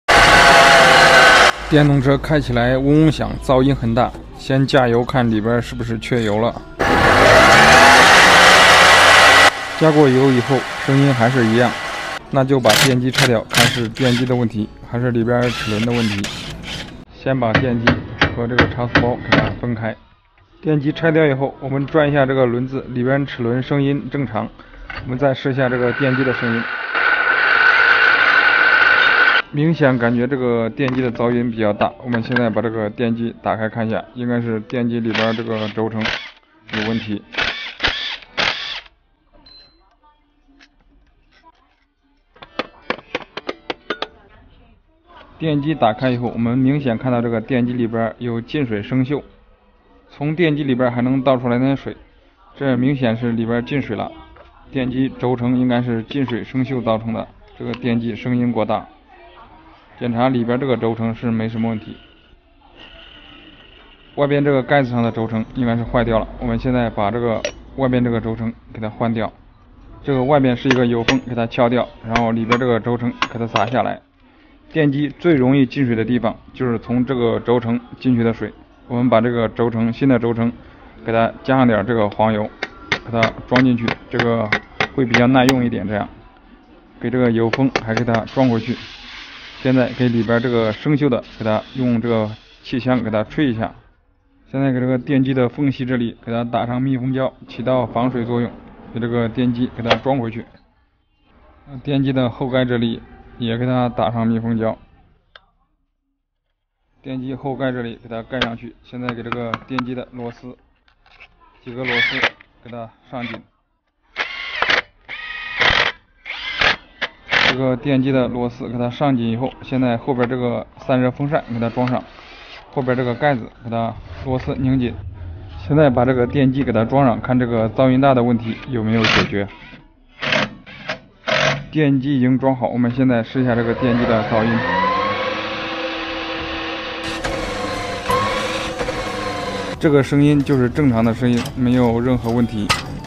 電動車三輪車噪音大不是缺油是軸承問題#電動車維修
從事電動車維修工作已經超過十年，這期間，我積累了豐富的經驗和技能，能夠有效地解決各種電動車故障。